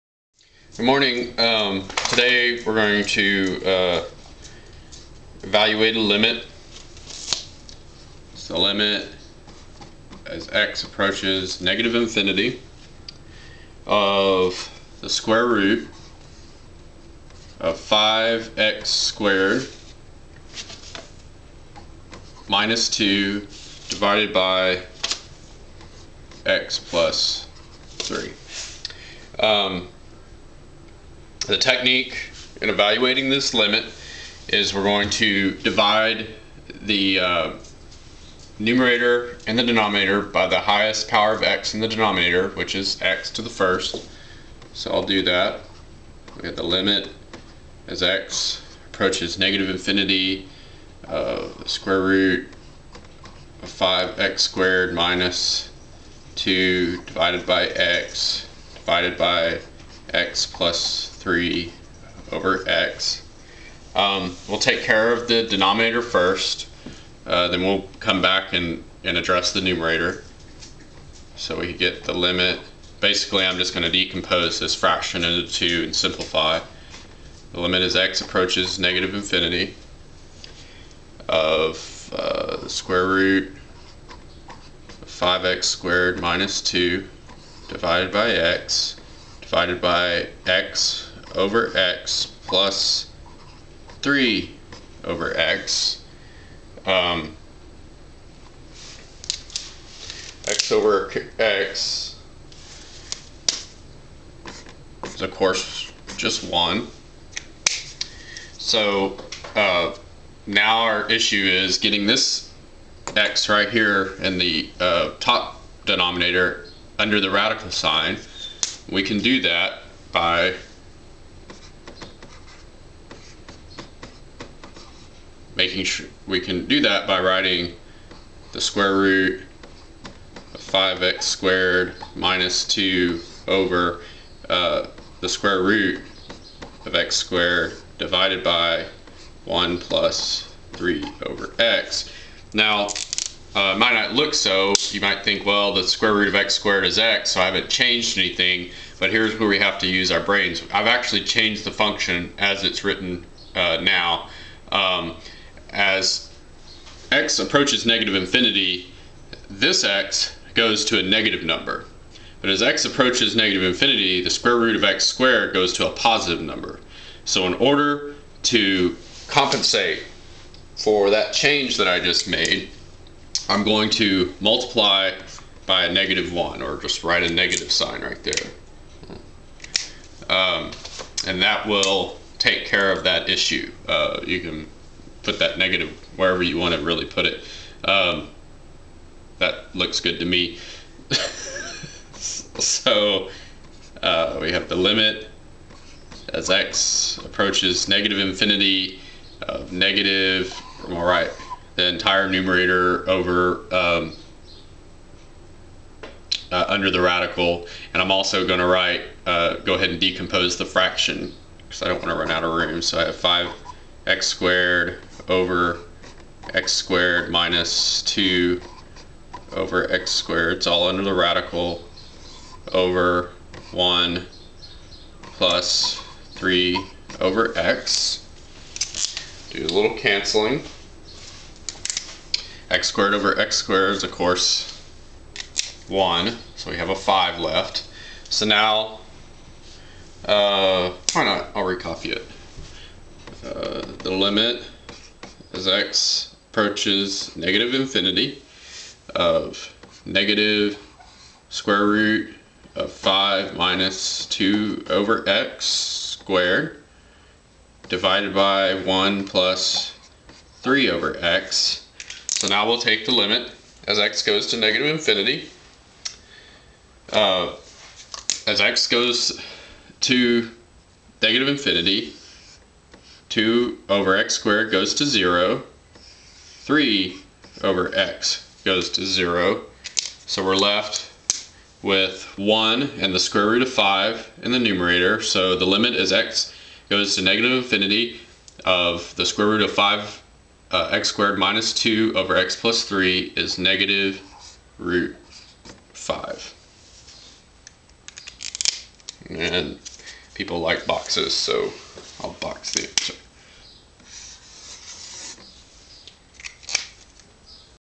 Limit as x approaches negative Infinity (1)
Evaluating a limit as x approaches negative infinity when a radical is in the numerator.

Here is a link to a solution to a limit submitted in a comment